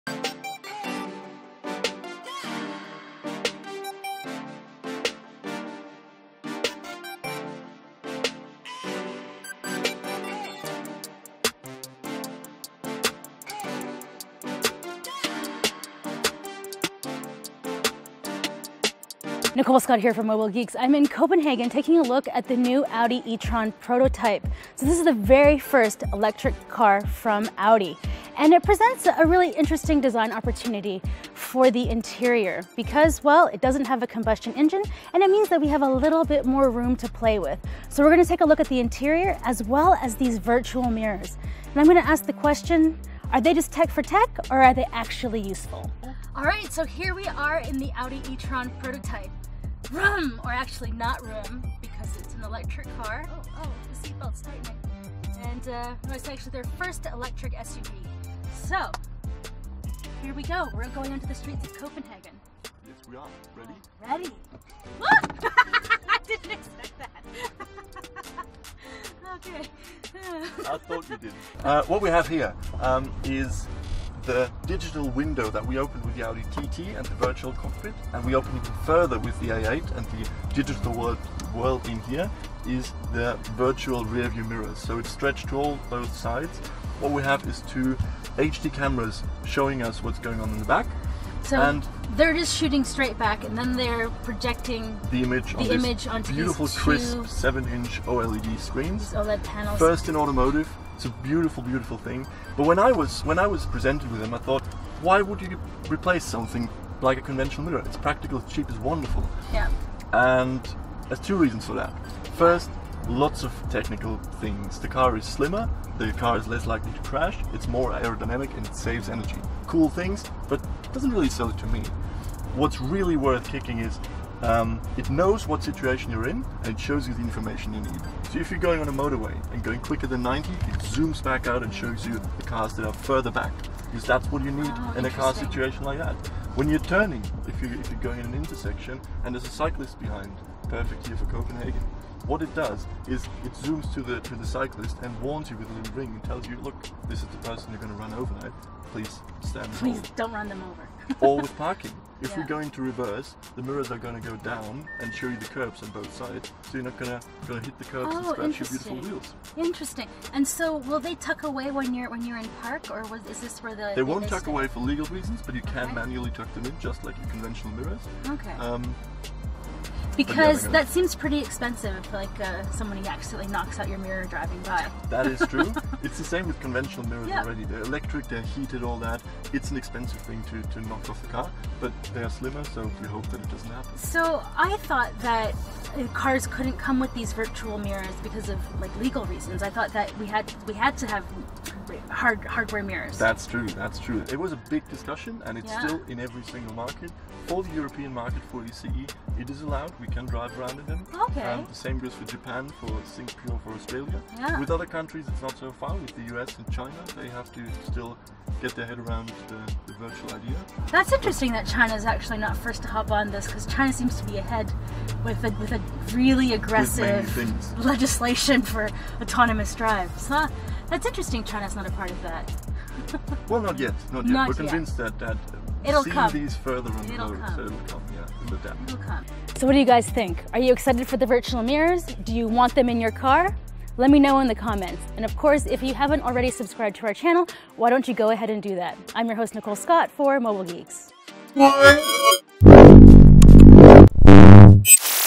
Do we need virtual mirrors? The Audi E-Tron Prototype Interior
The Audi E-Tron is technically a prototype but if you peel off the stickers it will suddenly turn into a production ready vehicle that will be hitting the streets by the end of the year. The design langugage that you'll find in this car will carry down through all of Audi's future electric vehicles so it's worth taking a deep dive on the E-Tron.

The E-Tron interior screams futuristic, but it's not so far out there that we can't imagine living in it. There are five screens surrounding you, the fully digital instrument cluster, the infotainment system, and the touchscreen mounted lower on the center console for the climate control settings as seen in high-end Audis like the A8, Q8, A7, and A6. The final two screen are OLED displays mounted in the doors showing acting as the virtual mirrors.